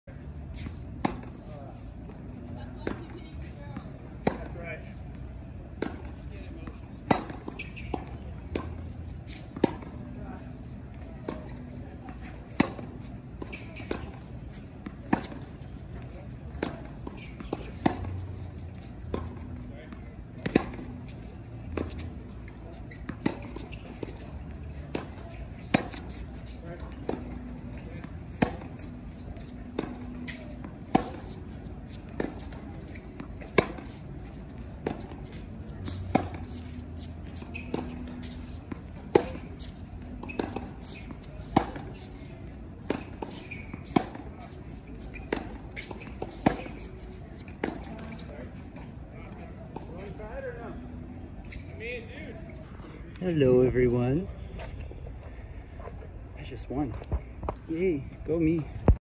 Hit with former ATP #20 Dmitry Tursunov
Hitting with ATP great and renowned pro coach, Dmitry Tursunov at the BNP Paribas Open.